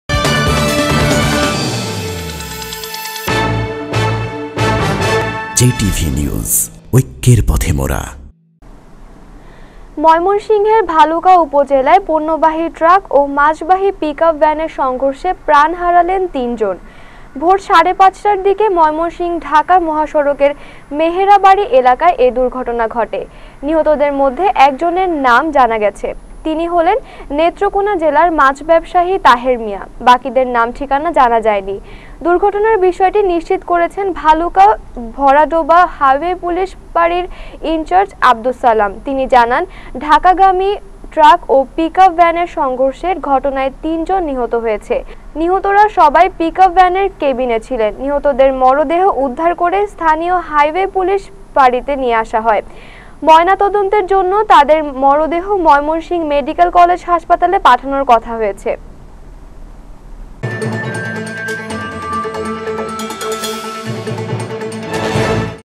JTV | ময়মনসিংহে ট্রাক-পিকআপ ভ্যান সংঘর্ষে ঝরল ৩ প্রাণ | JTV News
ময়মনসিংহের ভালুকায় উপজেলায় পণ্যবাহী ট্রাক ও একটি মাছবাহী পিকআপ ভ্যানের সংঘর্ষে প্রাণ হারালেন তিনজন। আজ সোমবার ভোর সাড়ে ৫টার দিকে ময়মনসিংহ-ঢাকা মহাসড়কের মেহেরাবাড়ি এলাকায় এ দুর্ঘটনা ঘটে।
নিহতদের মধ্যে একজনে নাম জানা গেছে। তিনি হলেন নেত্রকোণা জেলার মাছ ব্যবসায়ী তাহের মিয়া (৪৫)। বাকিদের নাম-ঠিকানা জানা যায়নি।
এ বিষয়টি নিশ্চিত করে ভালুকা-ভরাডোবা হাইওয়ে পুলিশ ফাঁড়ির ইনচার্জ আব্দুস সালাম গণমাধ্যমকে বলেন, ঢাকাগামী একটি ট্রাককে পেছন দিক থেকে মাছবাহী একটি পিকআপ ভ্যান ধাক্কা দেয়। ফলে পিকআপ ভ্যানটি ওই ট্রাকের ভেতরে ডুকে যায়। এতে পিকআপ ভ্যানটির কেবিনে থাকা চালক, হেলপার (সহকারী) ও মাছের মালিক নিহত হন। পরে তাদের মরদেহ উদ্ধার করে স্থানীয় হাইওয়ে পুলিশ ফাঁড়িতে নিয়ে আসা হয়েছে।
নাম-পরিচয় শনাক্তের পর ময়না-তদন্তের জন্য মরদেহ ময়মনসিংহ মেডিকেল কলেজ (মমেক) হাসপাতাল মর্গে পাঠানো হবে বলেও জানান তিনি।